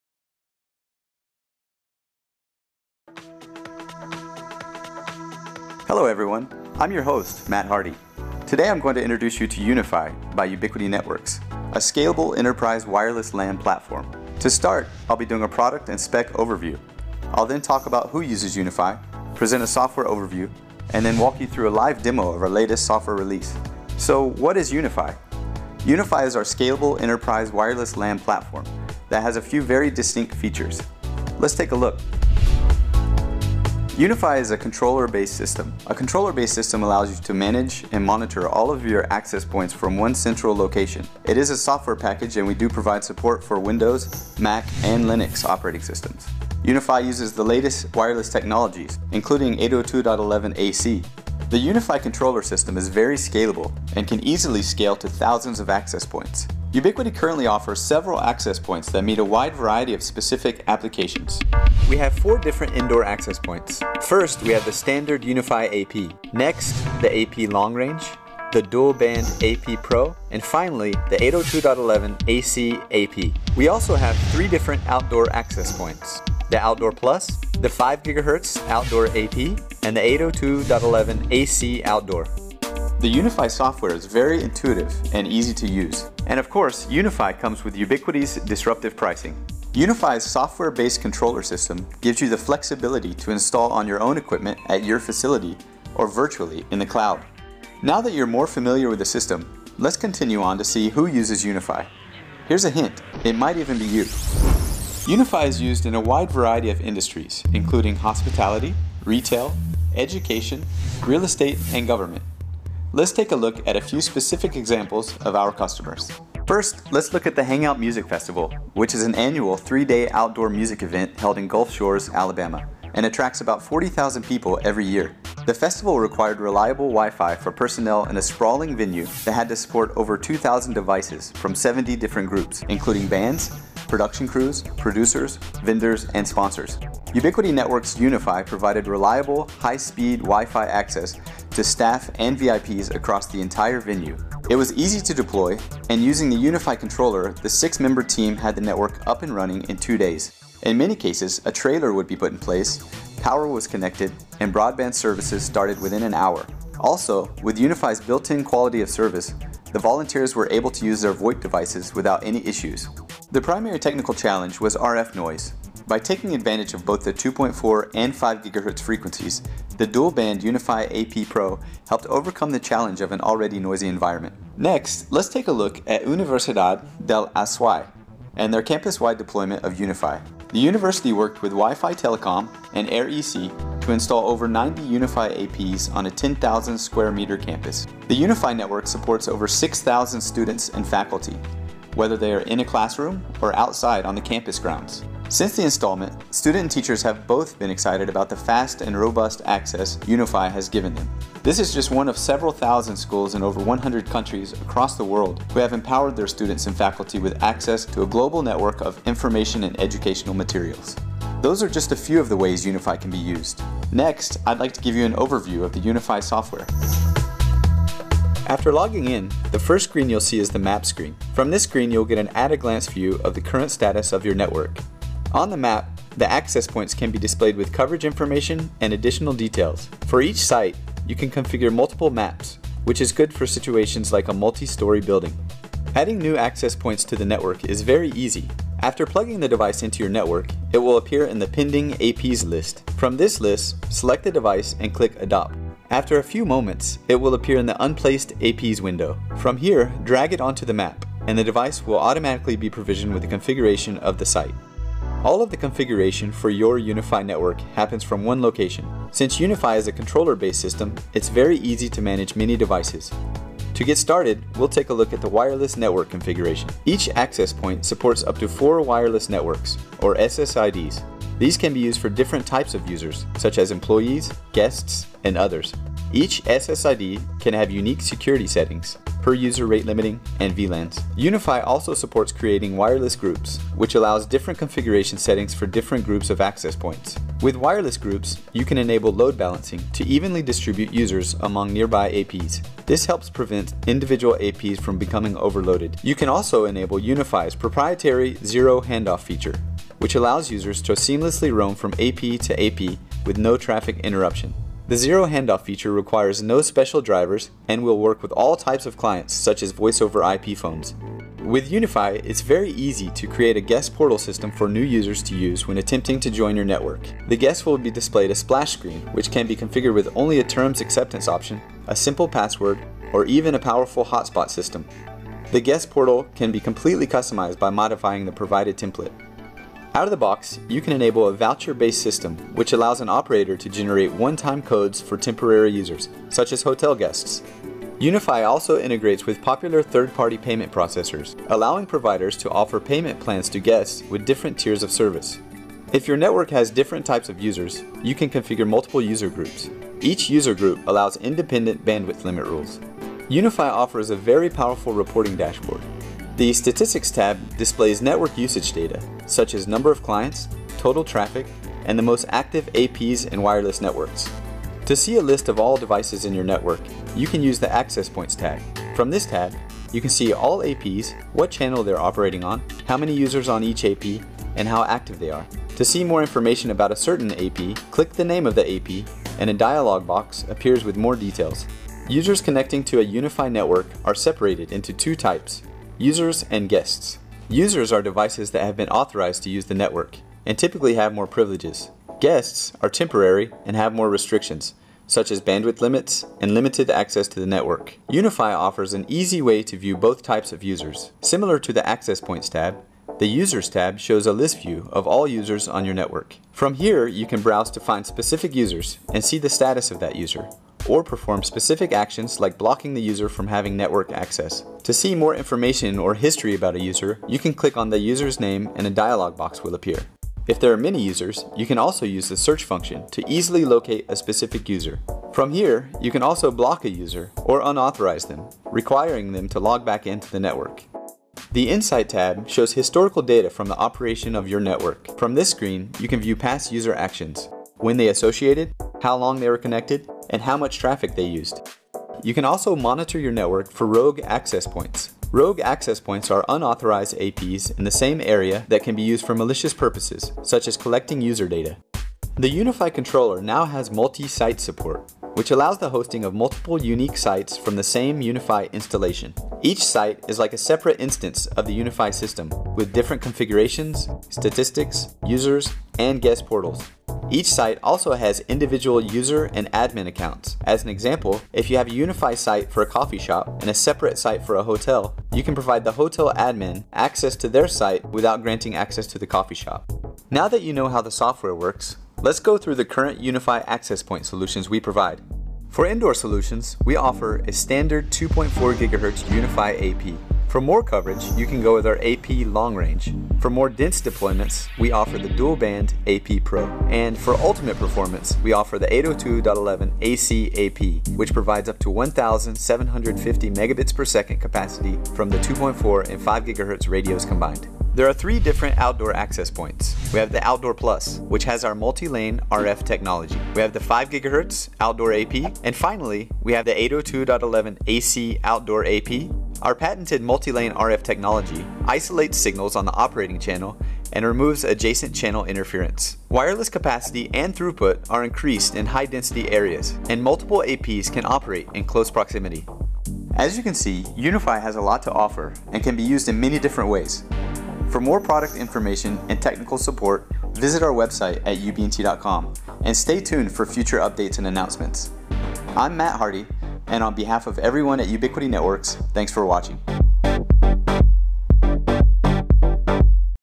Ubiquiti UniFi Introduction
An overview video for Ubiquiti's range of UniFi Access Point products